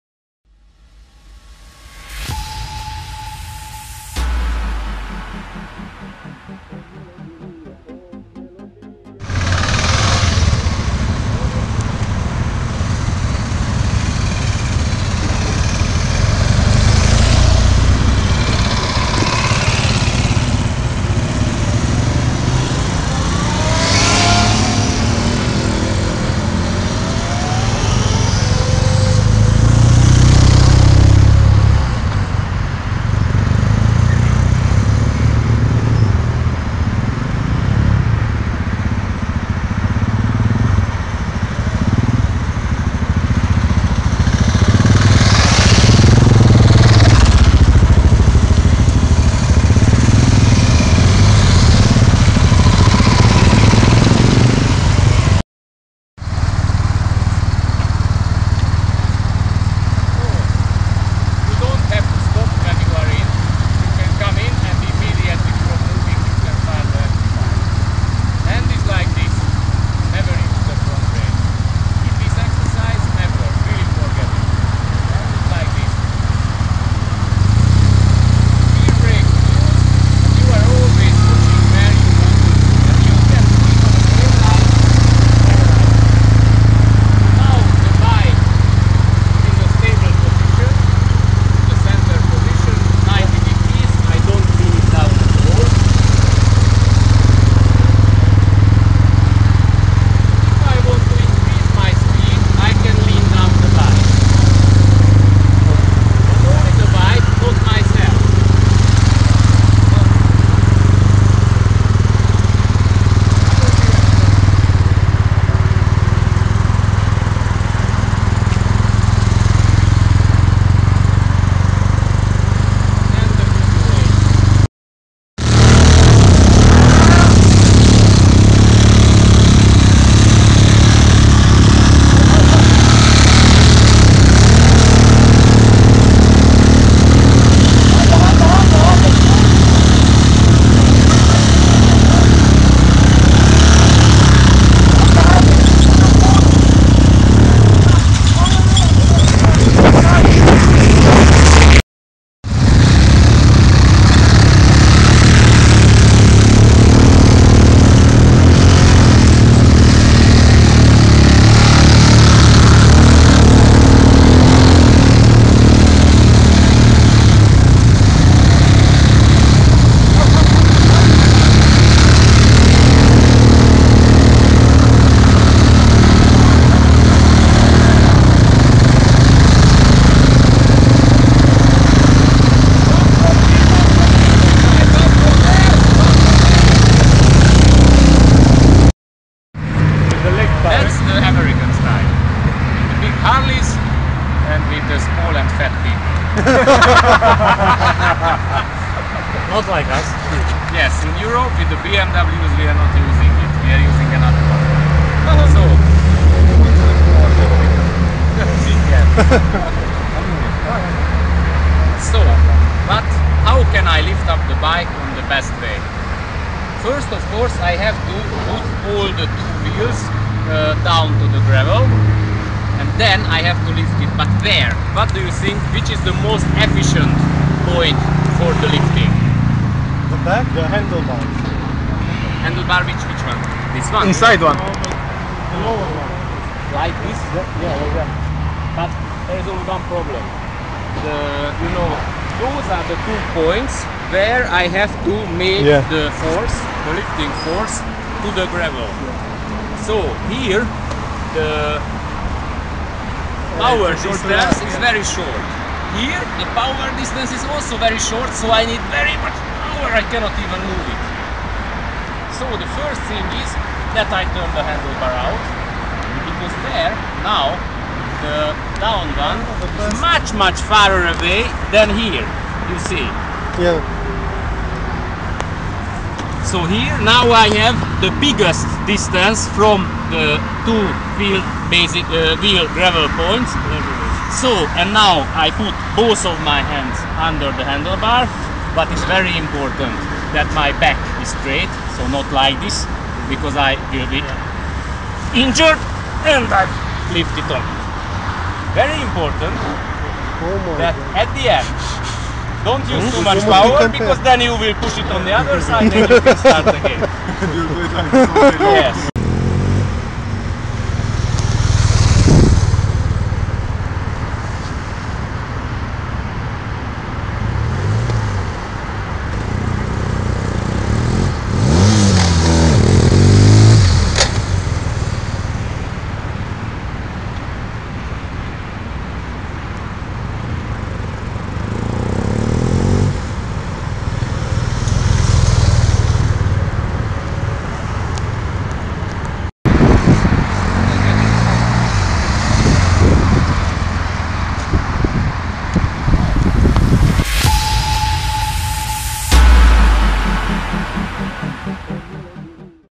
BMW Rider Training Bucuresti
Curs pilotaj moto, destinat motociclistilor cu experienta redusa sau cu o intrerupere mare in utilizarea motocicletei.